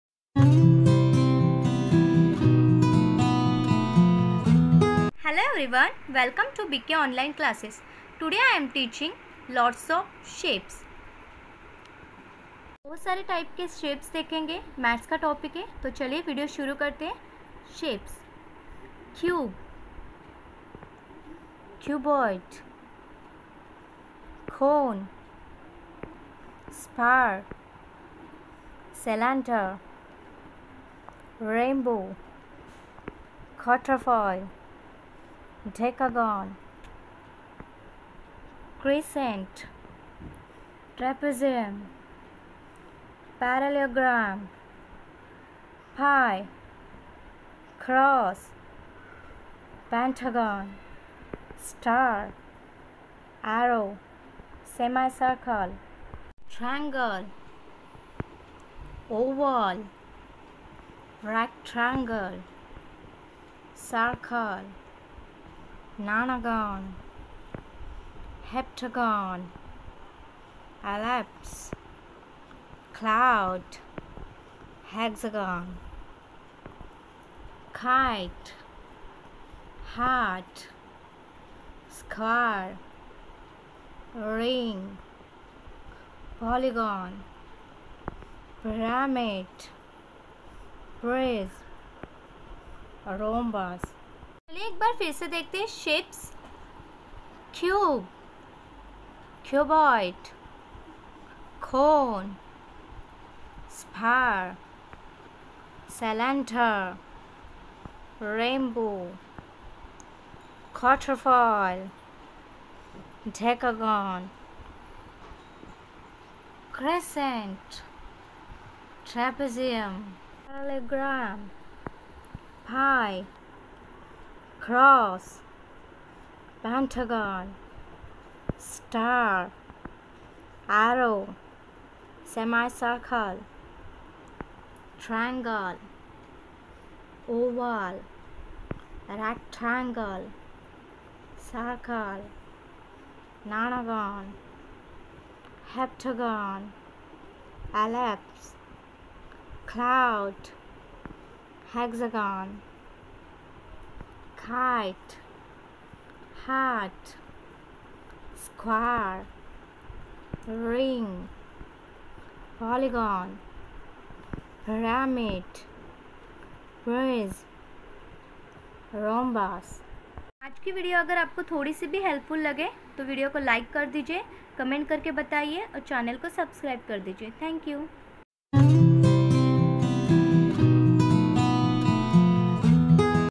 Math shapes/english vocabulary with pictures/for cbsc/Different types of shapes/how to teach shapes
In this video i am introduced lots of math shapes for cbsc/state board children/math topic here /improve English vocabulary/how to Draw shapes & how to learn those names here/learn spelling /lots of shape with pictures here /baccho ko shapes kese samjaye
Basic shapes
Bk online classes
Online with bk classes
Math shapes
English vocabulary improve